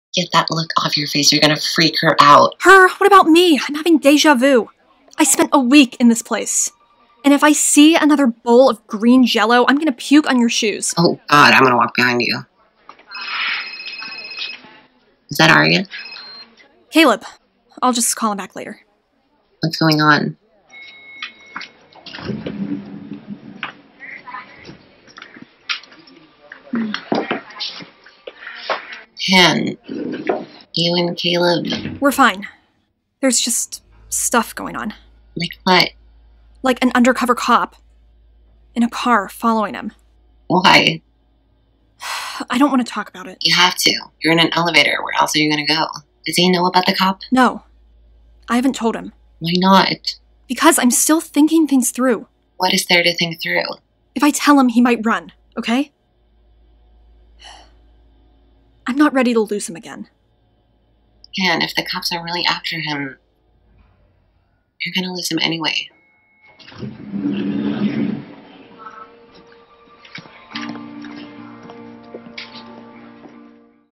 pll voiceover collab | "i'm not ready to lose him again"
How is it possible that I forgot all about this scene?! I'm so happy with how this collab turned out, thank you to Elias for joining as the Spencer to my Hanna and doing such an outstanding job!!

Spencer - CyberrCat
Hanna - me